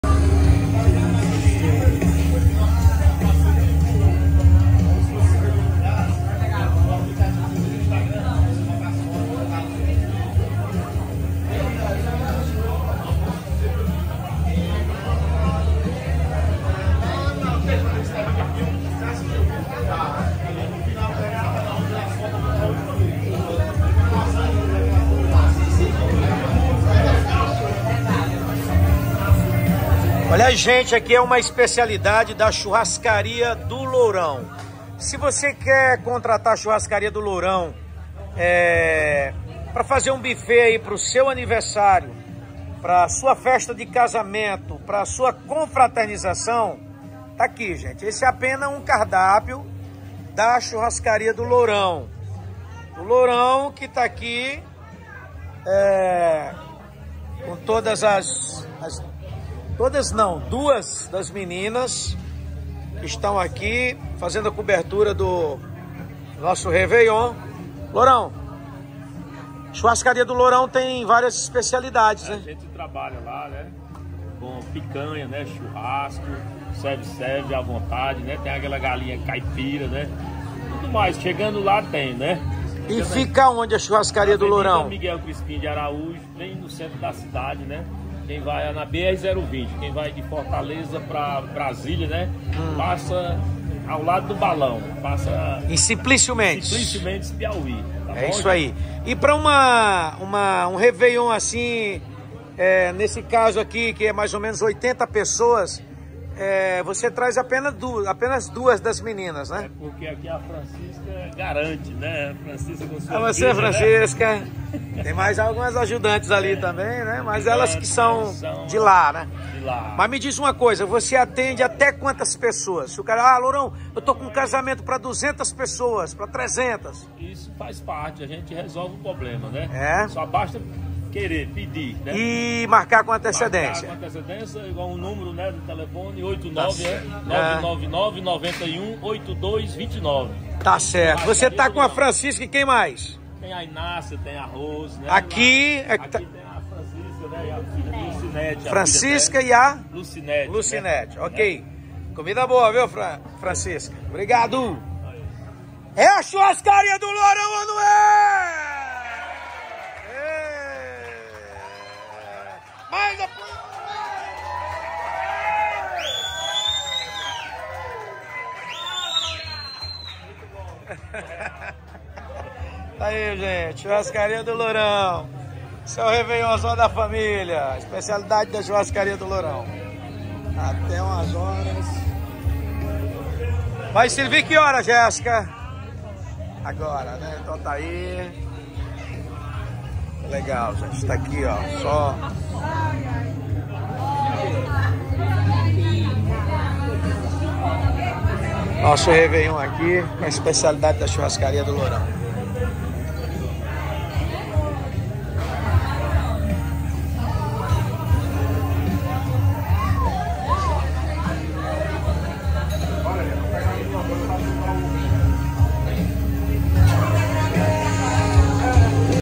CHURRASCARIA DO LOURÃO EM SIMPLÍCIO MENDES-PI | Francis Lopes Ao Vivo Na Estrada
Confira um pouco das variedades de PRATOS da CHURRASCARIA DO LOURÃO na
Av Miguel Crispin de Araújo, 577
Bairro: Nova Cidade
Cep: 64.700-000
Simplício Mendes-PI, Bem em frente ao balão da BR 020